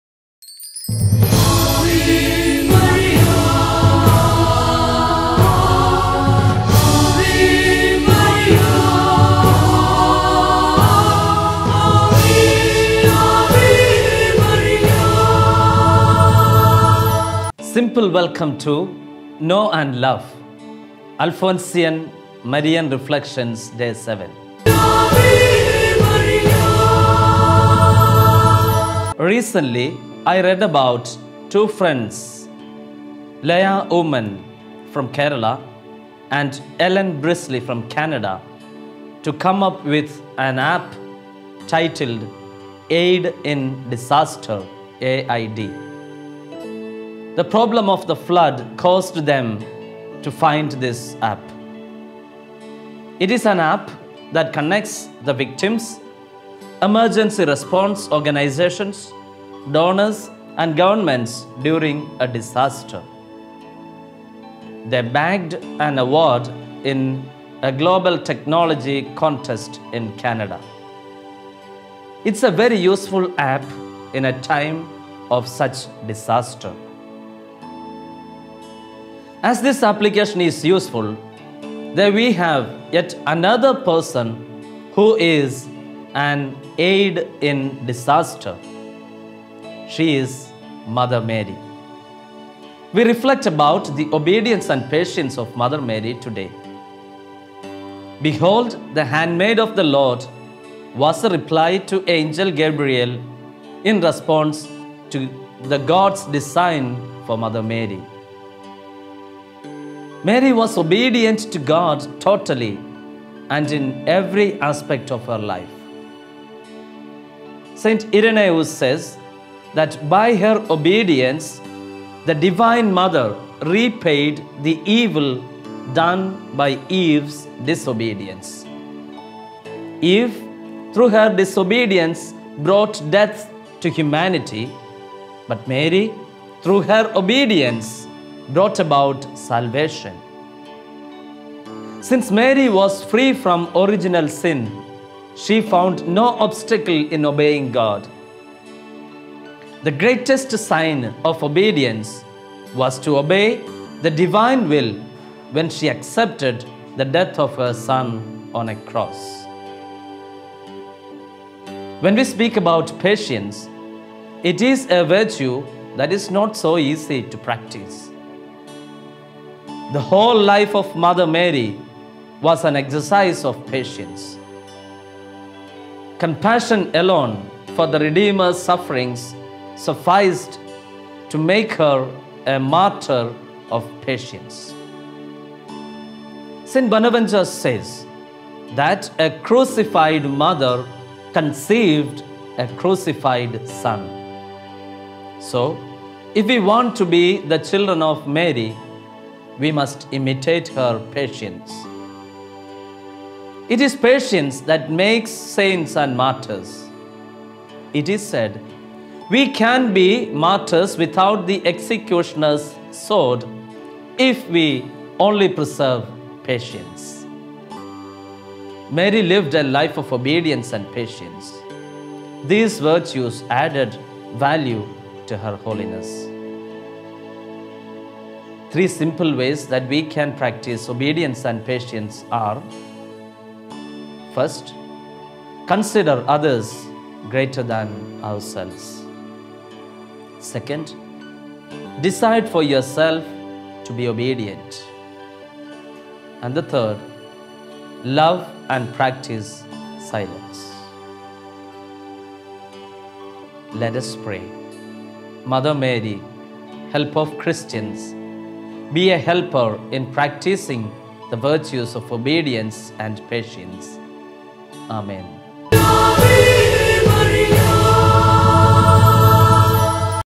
Alphonsian Marian Reflections Day 7 MARY’S OBEDIENCE AND PATIENCE
By Fr. Bijo Meppurathu CSsR
‘KNOW & LOVE’: Alphonsian Marian Reflections
DAY 7
MARY’S OBEDIENCE AND PATIENCE
Help of Christians
Simple welcome to ‘KNOW & LOVE’: Alphonsian Marian Reflections - Day 7
Recently I read about two friends Leya Oommen from Kerala and Ellen Brisley from Canada to come up with an app titled Aid in Disaster (A.I.D.). The problem of the flood caused them to find this app. It is an app that connects the victims, emergency response organisations, donors and governments during a disaster. They bagged an award in a global technology contest in Canada. It is a very useful app in a time of so much disaster.
As this application is useful, there we have yet another person who is an ‘Aid In Disaster’. She is Mother Mary. We reflect about the Obedience and Patience of Mother Mary.  “Behold the handmaid of the Lord’ was the reply to angel Gabriel in response to the God’s design for Mother Mary. Mary was obedient to God totally and in every aspect of her life. St. Iranaeus says that by her obedience, the divine Mother repaired the evil done by Eve’s disobedience. Eve through her disobedience brought death to humanity but Mary, through her obedience brought about salvation.  Since Mary was free from original sin, she found no obstacle in obeying God. The greatest sign of obedience was to obey the divine will when she accepted the death of her son on a Cross.
When we speak about Patience, it is a virtue that is not so easy to practice. The whole life of Mother Mary was an exercise of patience. Compassion alone for the Redeemer’s sufferings sufficed to make her a martyr of patience. St. Bonaventure says, “That a crucified Mother conceived a crucified Son”. So if we want to be the children of Mary, we must imitate her patience. It is patience that makes saints and martyrs. It is said, ‘we can be martyrs without the executioner’s sword, if we only preserve patience’. Mary lived a life of obedience and patience. These virtues added value to her holiness and spirituality.
3 simple ways that we can practice Obedience and Patience are
1. Consider others greater than ourselves.
2. Decide for yourself to be obedient.
3. Love and practice silence.

Let us Pray: Mother Mary, Help of Christians, be a helper in practicing the virtues of obedience and patience. Amen.